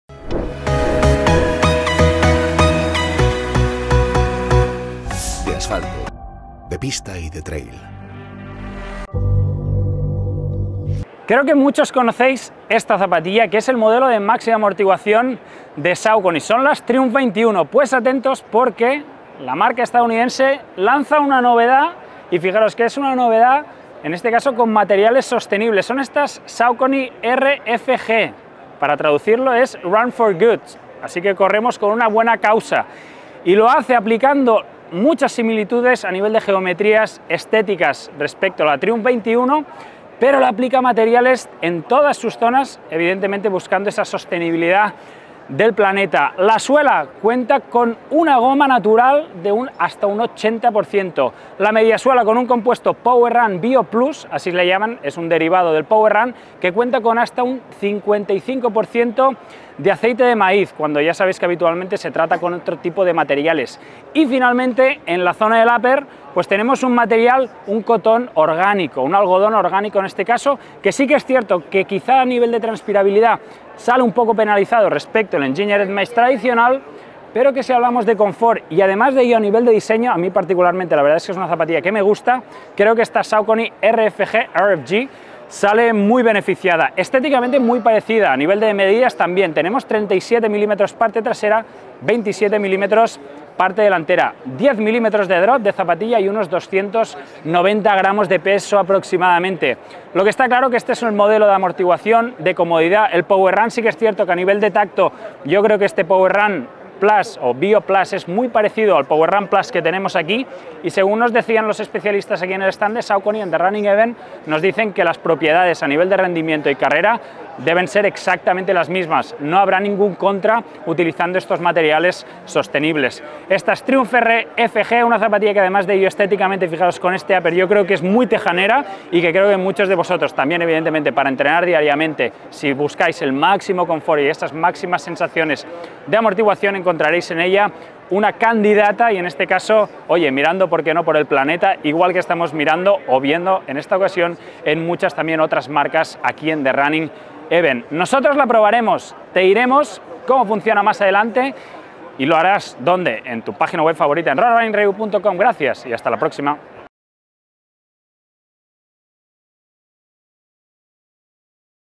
Saucony Triumph RFG | Una de las grandes del mercado se vuelve "totalmente" sostenible
Consigue un 5% en la compra de cualquier zapatilla de Top4running a través de este link aplicando el código RRR :

El cuidado medioambiental y el uso responsable de materiales en el proceso de fabricación de las zapatillas de running está cada vez más presente en el mercado y las marcas son más conscientes de ello.

Esta vez ha sido Saucony quien ha lanzado o lanzará próximamente una versión sostenible de su modelo más emblemático, la Triumph.

Se trata de la nueva Saucony Triumph RFG (Run for Good), que, además de mantener las propiedades y el espíritu rodador del modelo, aplica materiales reciclados en todas y cada una de sus partes.

Aunque de buenas a primeras la zapatilla gana enteros como zapatilla “casual” o de calle, los propios técnicos de la marca nos aseguraron que sería igual de eficiente en carrera que la Triumph 21 actual, con la única diferencia, que el material utilizado para desarrollarla proviene de otros compuestos reutilizados.

En la mediasuela tenemos el PwrRun Bio+, un nuevo material para la marca que cuenta con hasta un 55% de aceite de maíz. El upper está fabricado a través de algodón orgánico, que, si bien por su rigidez y densidad penalizará probablemente la transpirabilidad, en cuanto a confort y comodidad (además de diseño) creemos que puede gustar y mucho.

Y para finiquitar con las tres partes principales de la Triumph RFG, nos queda repasar la suela. Una suela con un 80% de goma natural con un diseño idéntico al de la Triumph 21 pero con un colorido más translúcido y neutro.

TIPO DE CORREDOR:

Si de por sí ya eras fan de la Triumph, esta versión RFG es muy probable que esta atípica versión se convierta en tu próxima adquisición.

Por su estética y diseño, puede que encaje un poco más en la vestimenta del día a día que para darle un uso focalizado en el rendimiento, pero oye, como opción eco de máxima amortiguación, incluso para rodar suave, tenemos que reconocer que la iniciativa nos gusta.

Mismos niveles de amortiguación, compuestos parecidos a los de la Triumph 21 y como cabe de esperar, toda la protección más que necesaria para sentir todo lo característico del modelo neutro de los estadounidenses.

DISTANCIA:

La Saucony Triumph RFG es una zapatilla de máxima amortiguación con la que podrás correr prácticamente todo tipo de distancias, inclusive las más largas.